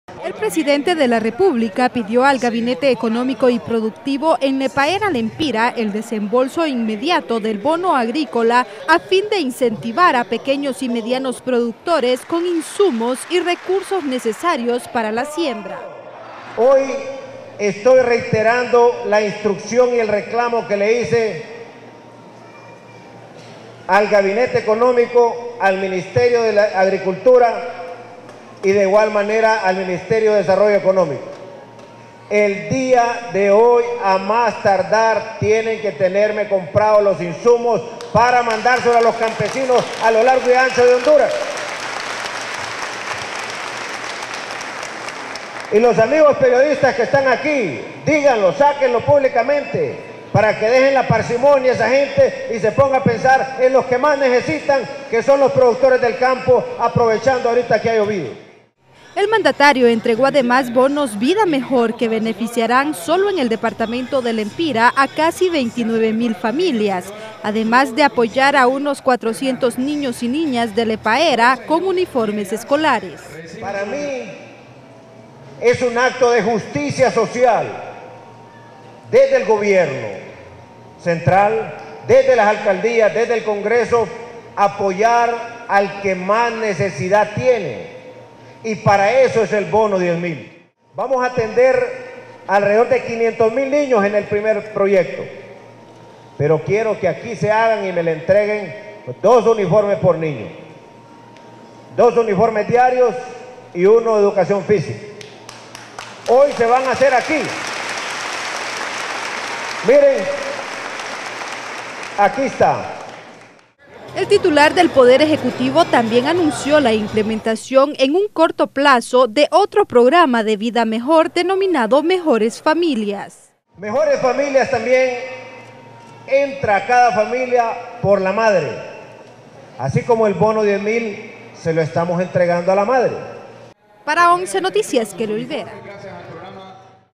El presidente pide al Gabinete Económico y Productivo desembolsar el pago del bono agrícola
El presidente de la república, pidió al gabinete económico y productivo en Lepaera, lempira,  el desembolso inmediato del bono agrícola a fin de incentivar a pequeños y medianos productores con insumos y recursos necesarios para la siembra.

El mandatario entregó, además, bonos vida mejor que beneficiarán solo en el departamento de lempira a casi 29 mil familias, además de apoyar a unos 400 niños y niñas de Lepaera con uniformes escolares.

El titular del poder ejecutivo también anunció la implementación en un corto plazo de Otro programa de vida mejor denominado Mejores familias.